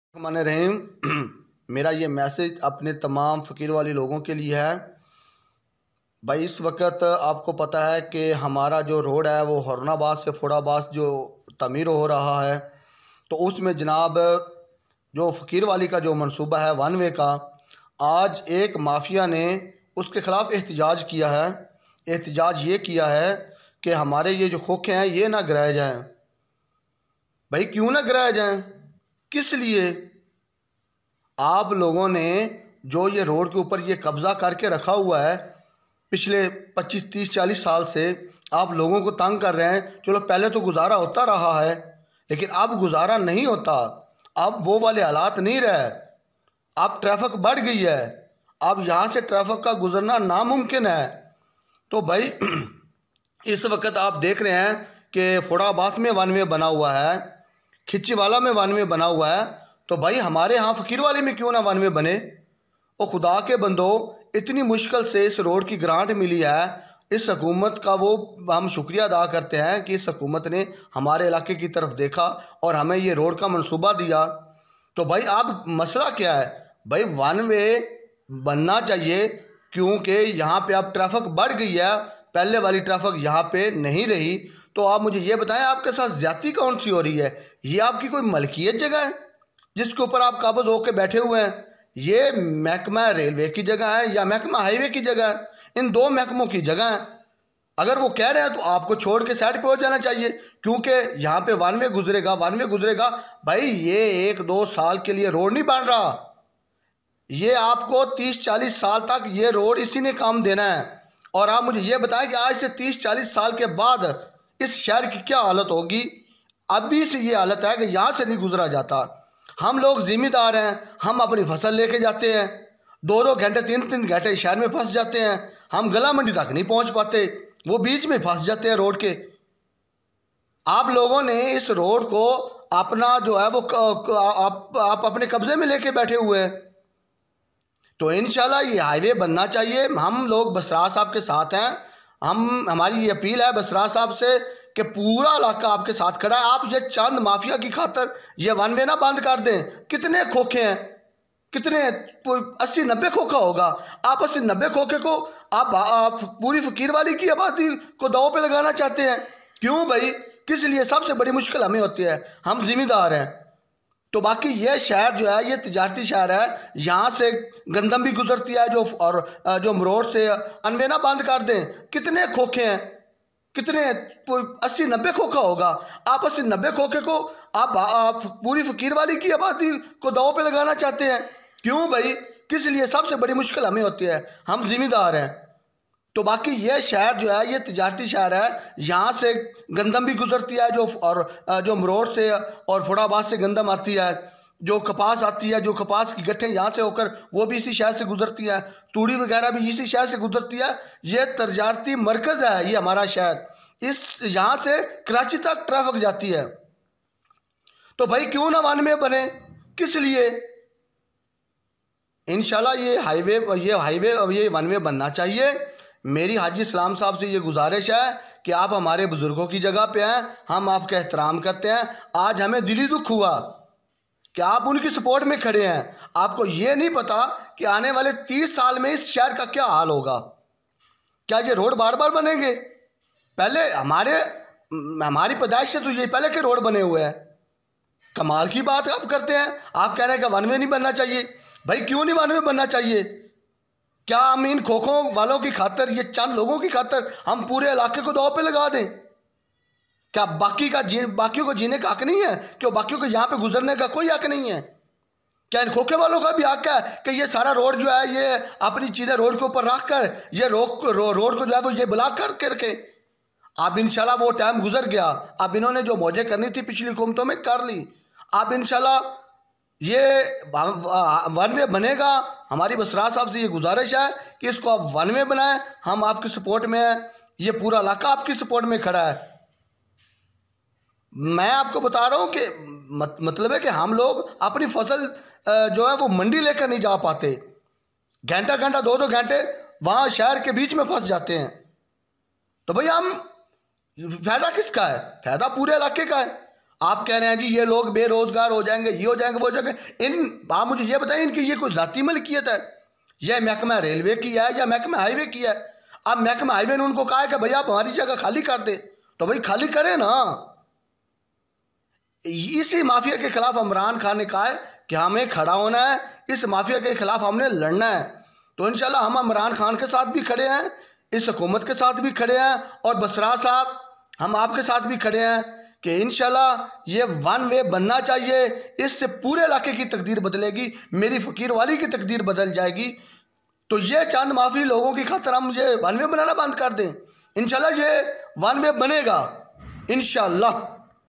Citizen's message on the issue of Haroonabad Road, Faqirwali One Way Road from Fort Abbas | 39 News
39 News brings you to live coverage of everything you need to know across business, finance, technology, politics, and more.